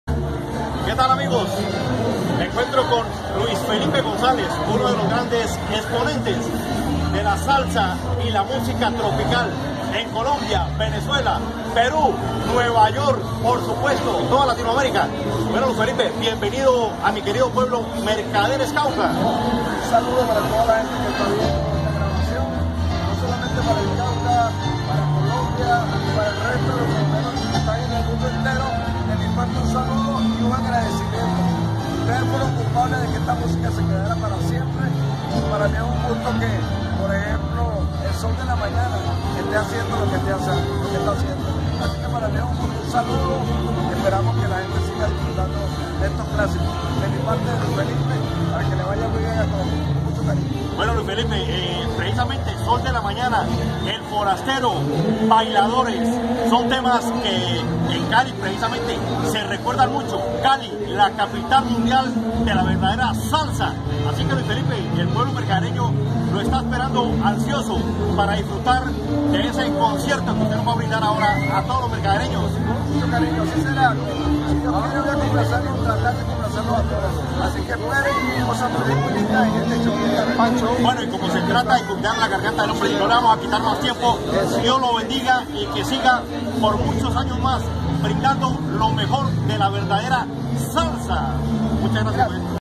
LUIS FELIPE GONZÁLEZ. Entrevista ENRIQUE ORDÓÑEZ.
Que tal amigos(a) Esta entrevista al maestro LUIS FELIPE GONZÁLEZ se realizó el 7 de enero de 2.020 en el cierre de los carnavales en mi querido pueblo MERCADERES Cauca Colombia, minutos antes de comenzar su presentación.